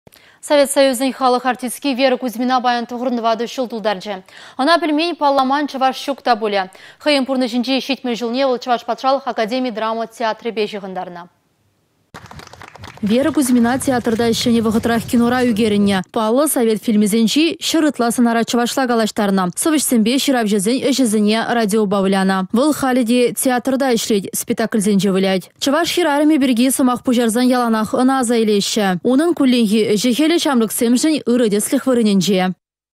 Вера Кузьмина паян 94 çул тултарчĕ.
Совет Союзĕн халăх артистки Вера Кузьмина паян 94 çул тултарчĕ. Ăна пĕлмен, палламан чăваш çук та пулĕ. Хăйĕн пурнăçĕнчи 70 çулне Чăваш патшалăх академи драма театрĕпе çыхăнтранă вăл.